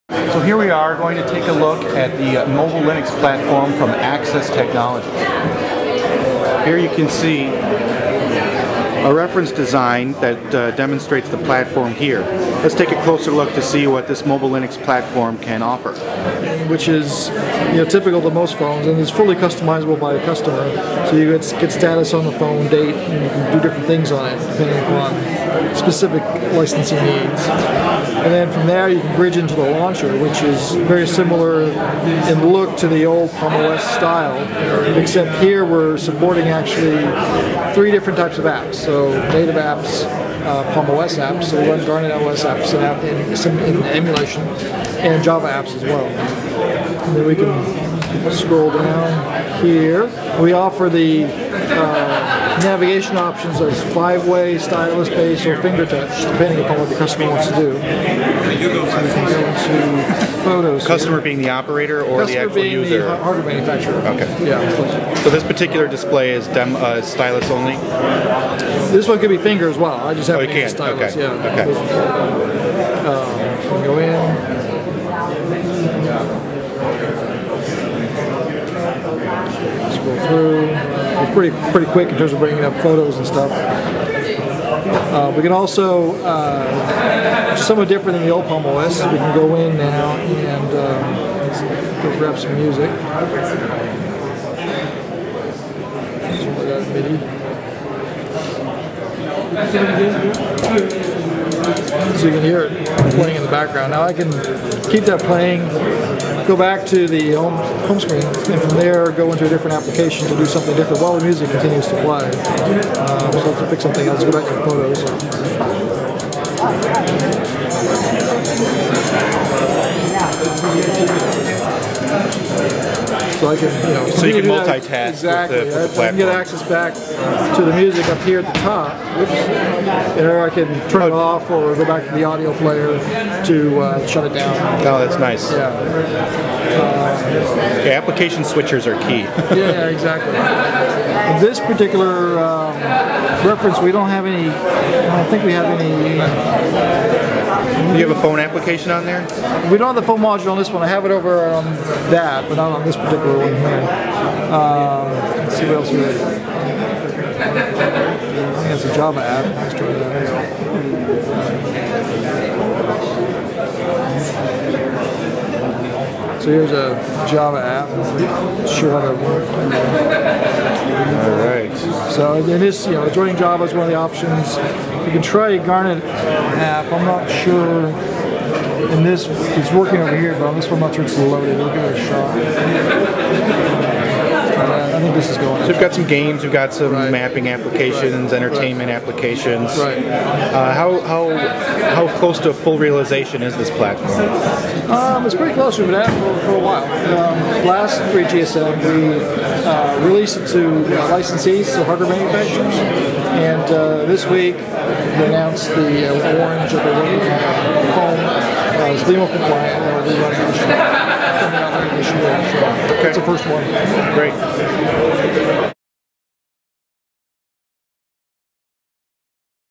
Access Mobile Linux
Here is a look at the mobile Linux platform form Access